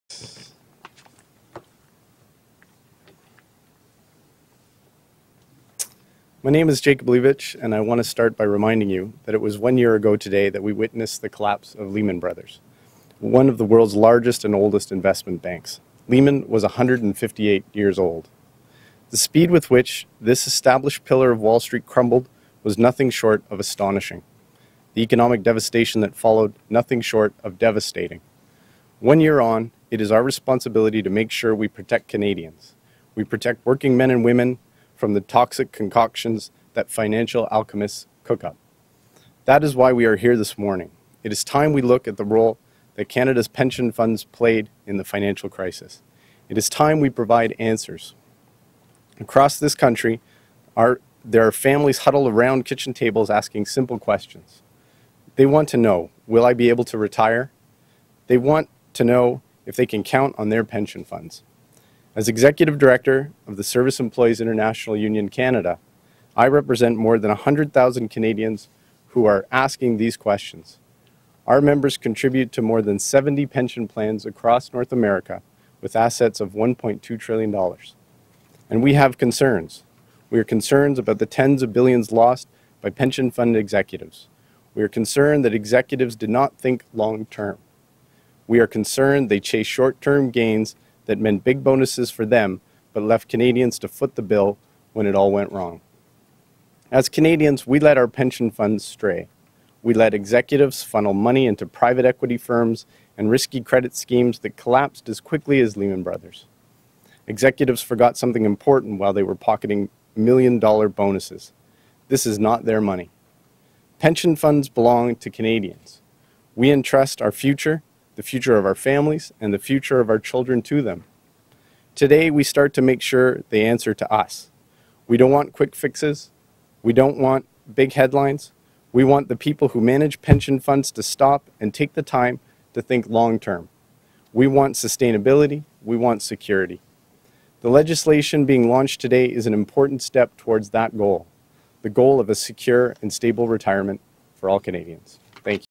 Jacob Leibovitch, SEIU, ethics for pension investments - 090915
Jacob Leibovitch, Executive Director of SEIU-Canada, speaks at a news conference on Parliament Hill in support of a private members' bill from Rob Oliphant, Liberal MP for Don Valley West, on ethical standards for pension fund investment - recorded by Ish Theiheimer for Straight Goods News, September 15, 2009.